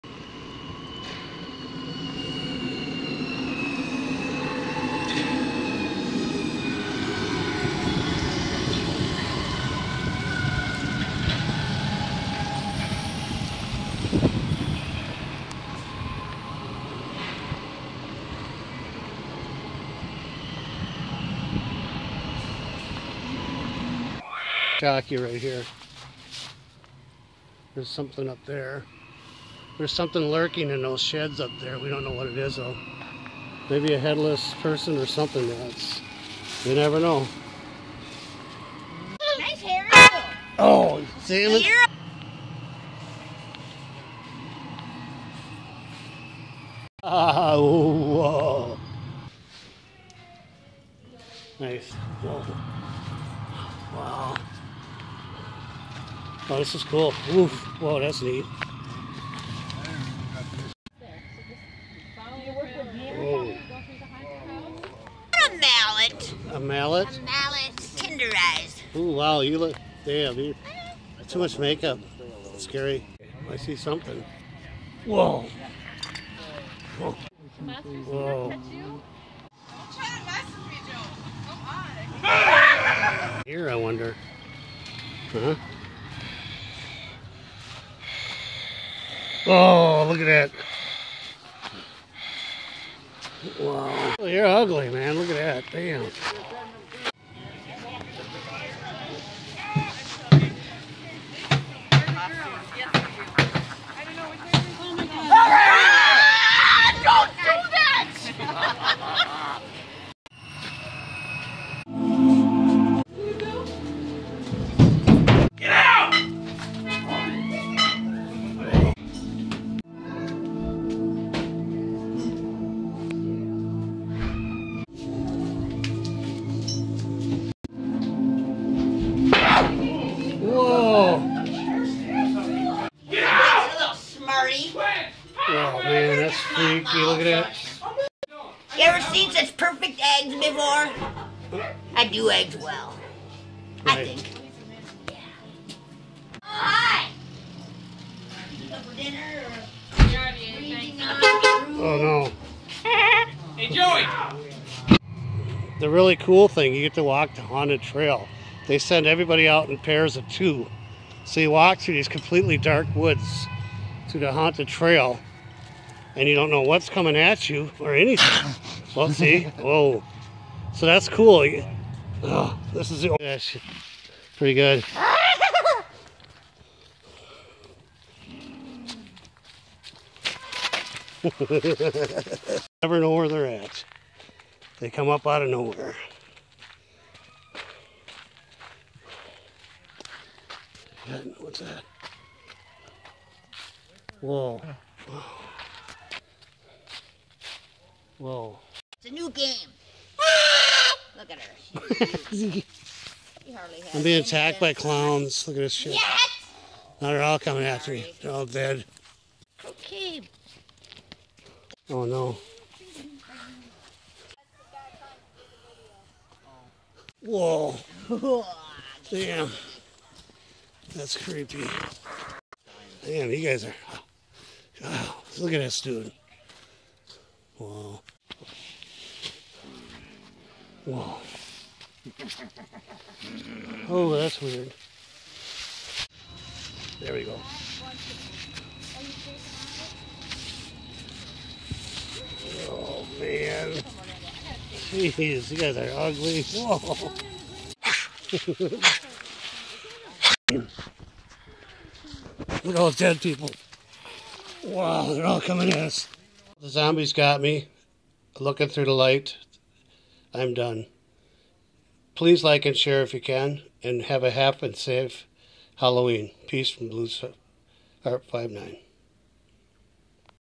Haunted Halloween - A Scary Campground.
Haunted Halloween - A Scary Campground. I filmed this video on Halloween 2016 with my friend at a Halloween haunt where he worked.